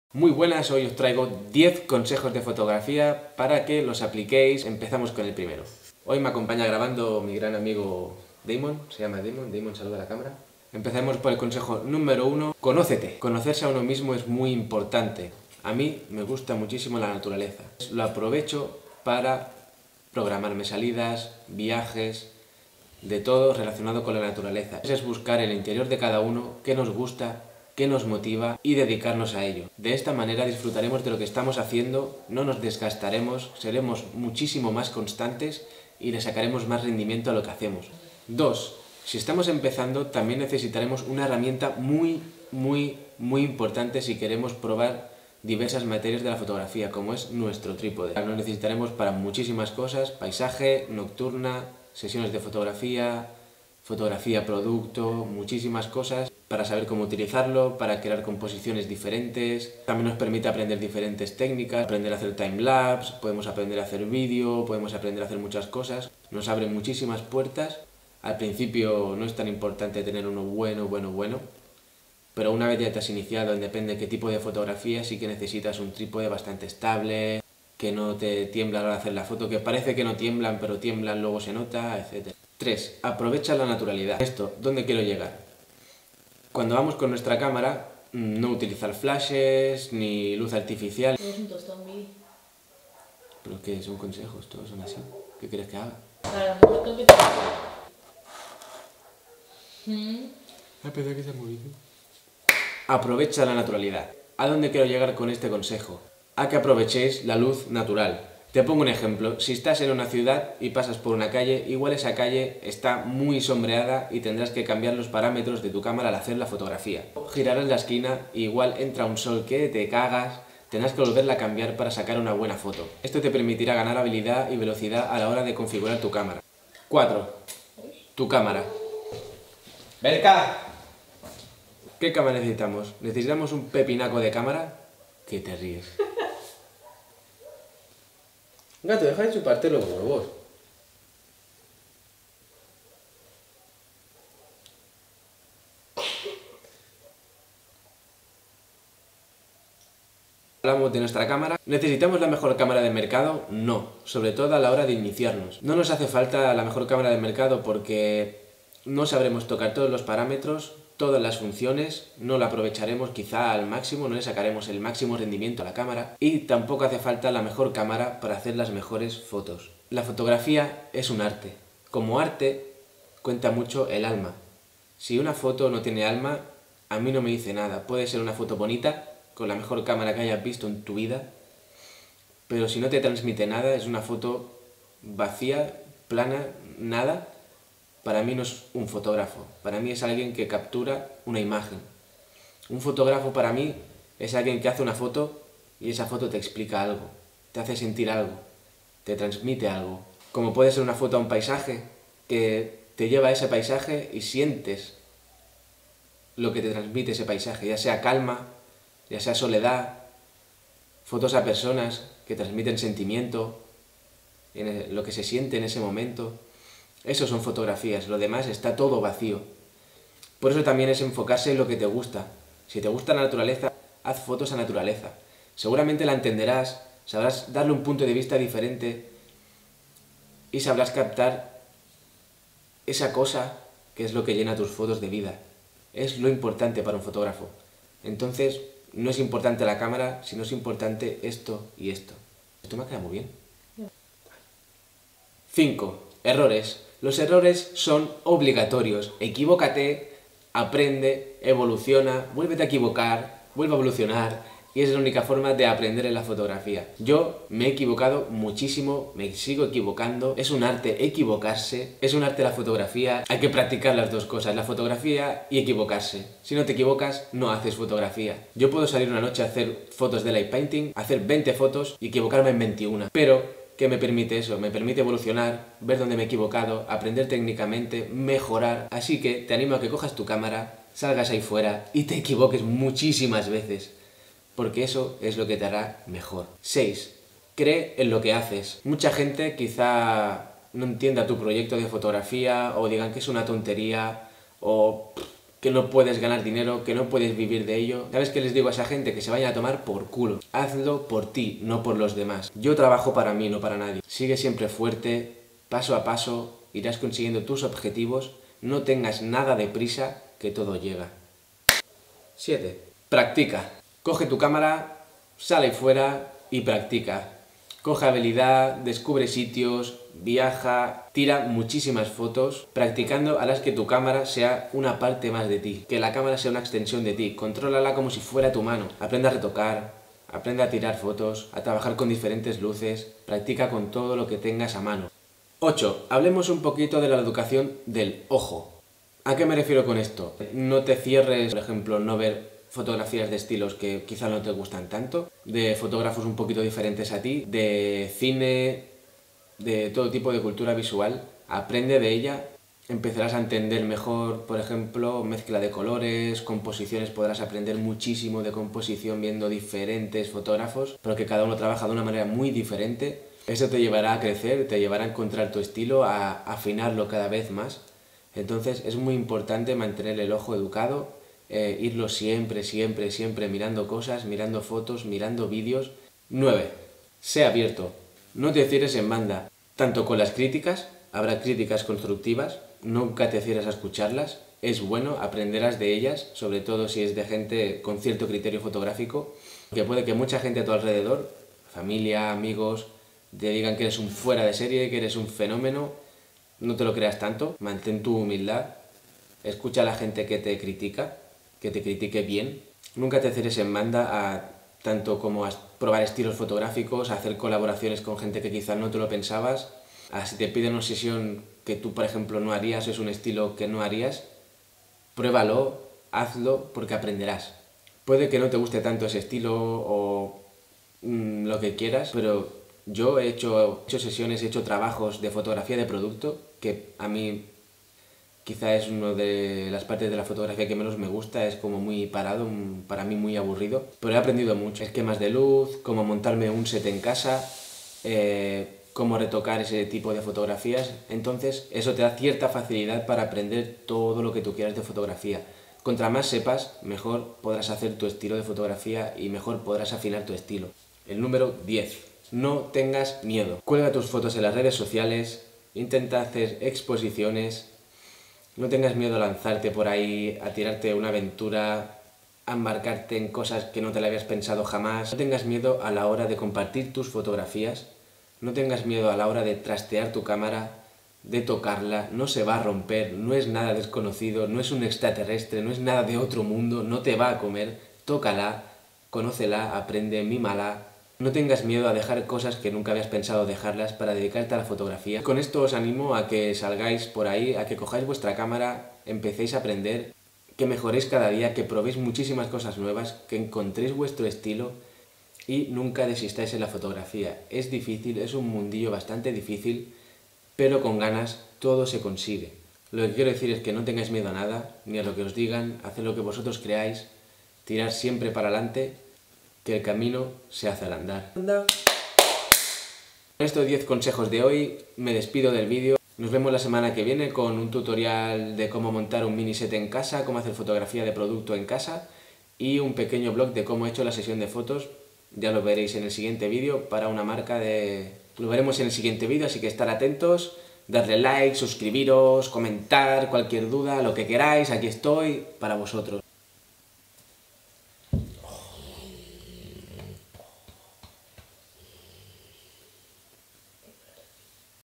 10 TIPS sobre FOTOGRAFIA para (PRINCIPIANTES) y no tanto!
En este vídeo os dejo 10 interesantes consejos, para poder evolucionar tanto técnicamente, en la forma de ver las cosas, en definitiva hacer un poco más fácil el largo camino del fotógrafo.

10 tips de fotografía para principiantes y no tanto, un intento de echar una mano a quien esté empezando y quizás recordar algún ítem a los que ya tienen experiencia.

Próximamente - Que objetivo me compro y con cual empezar?